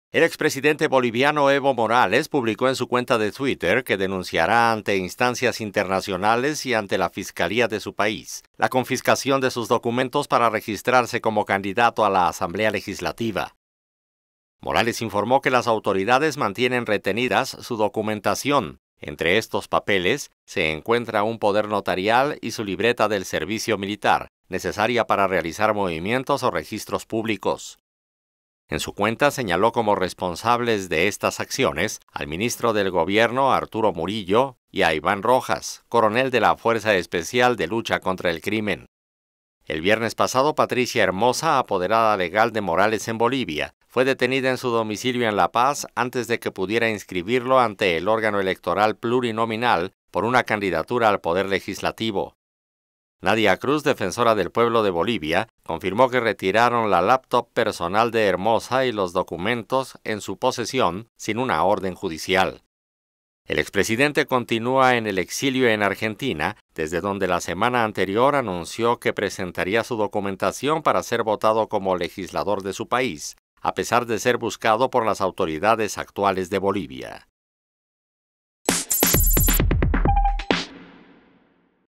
Evo Morales denuncia retención de documentos en Bolivia
El expresidente boliviano EvoMorales publicó en su cuenta de Twitter que denunciará ante instancias internacionales y ante la fiscalía de su país la confiscación de sus documentos para registrarse como candidato a la Asamblea Legislativa.